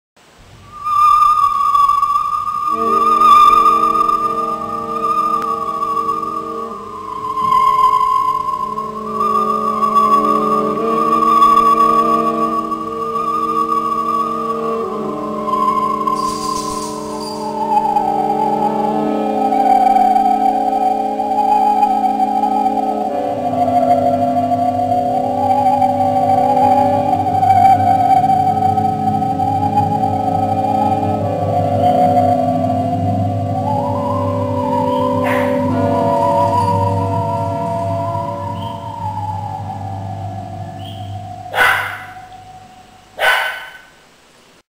This Old Lamp
The other day someone left an old chandelier on the corner of my street. Here's an eerie yet hopeful story of the lamp (upon being rescued and restored)!